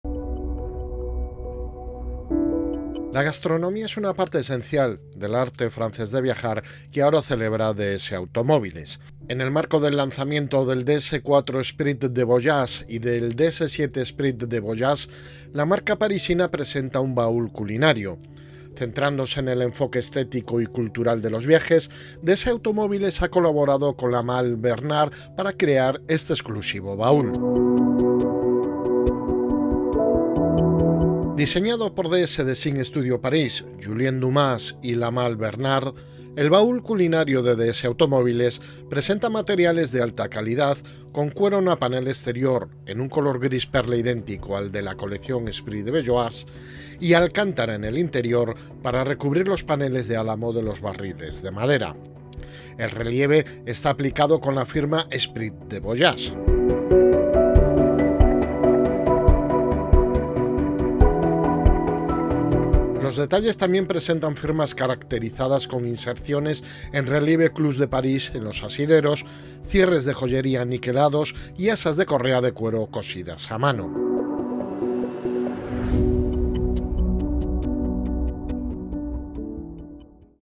UN BAÚL CULINARIO: EL ARTE FRANCÉS DE VIAJAR POR DS AUTOMOBILES | By CdRas
La gastronomía es una parte esencial del arte francés de viajar, que ahora celebra DS Automobiles. En el marco del lanzamiento del DS 4 ESPRIT DE VOYAGE y del DS 7 ESPRIT DE VOYAGE, la marca parisina presenta un baúl culinario. Centrándose en el enfoque estético y cultural de los viajes, DS Automobiles ha colaborado con La Malle Bernard para crear este exclusivo baúl.
Diseñado por DS DESIGN STUDIO PARÍS, Julien Dumas y La Malle Bernard, el baúl culinario de DS Automobiles presenta materiales de alta calidad con cuero napa en el exterior, en un color gris perla idéntico al de la colección ESPRIT DE VOYAGE y Alcantara en el interior para recubrir los paneles de álamo de los barriles de madera. El relieve está aplicado con la firma ESPRIT DE VOYAGE.
Los detalles también presentan firmas caracterizadas con inserciones en relieve Clous de París en los asideros, cierres de joyería niquelados y asas de correa de cuero cosidas a mano.